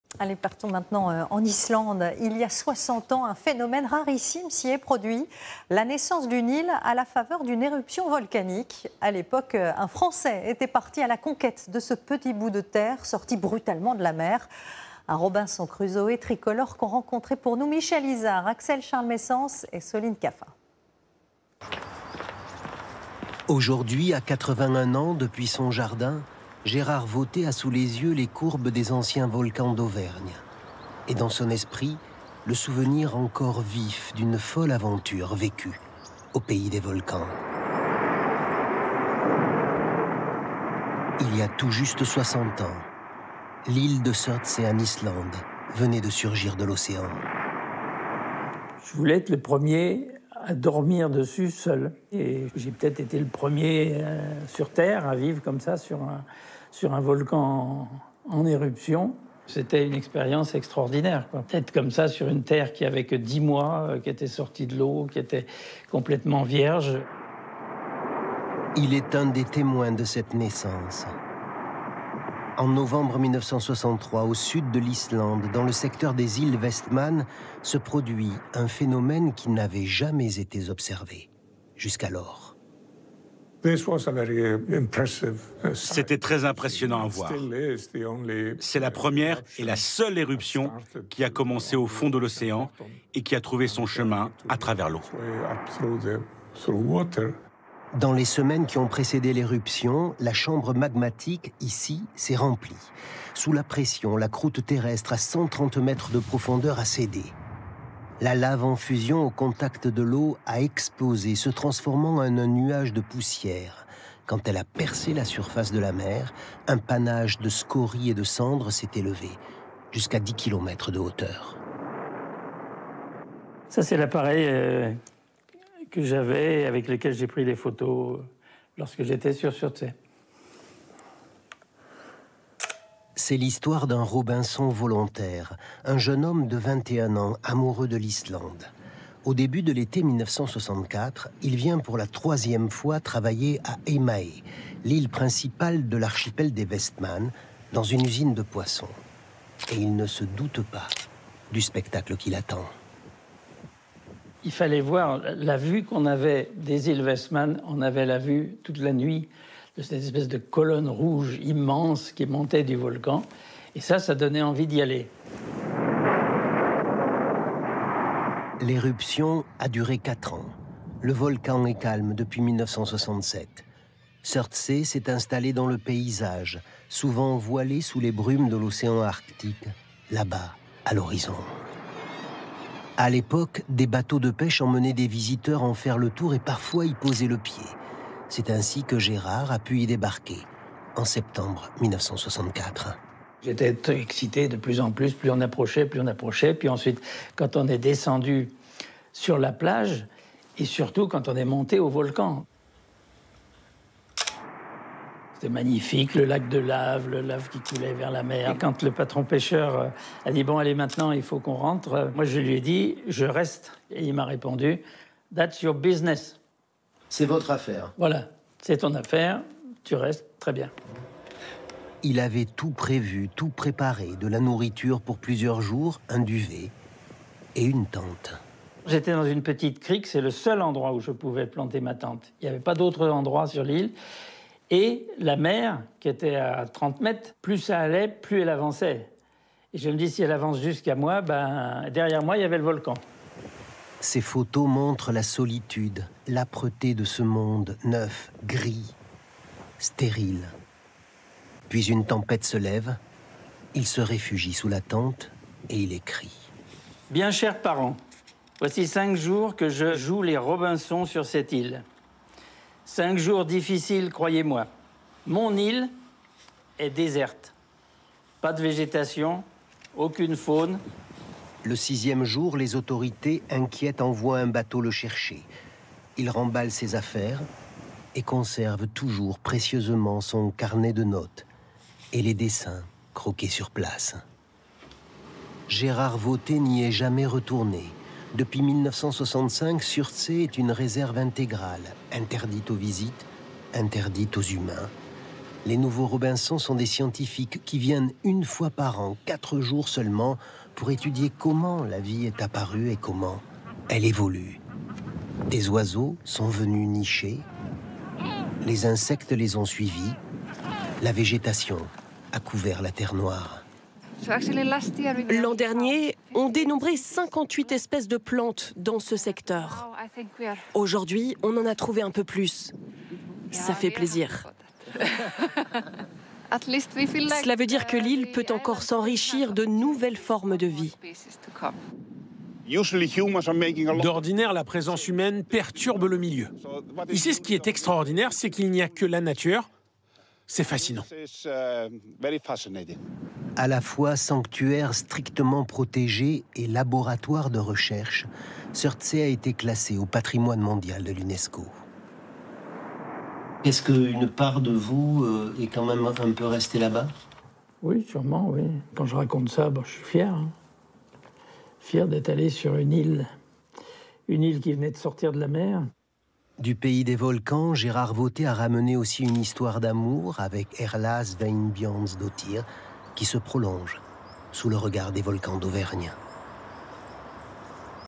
Islande - Camper sur un volcan : l'incroyable histoire d'un passionné Français｜TF1 INFO
En 1963, une nouvelle île est née au large de l'Islande, à la faveur d'une éruption volcanique. À l'époque, un Français est parti découvrir ce nouveau bout de terre, fasciné par ce phénomène rarissime. Soixante ans plus tard, Gérard Vautey raconte son périple à TF1.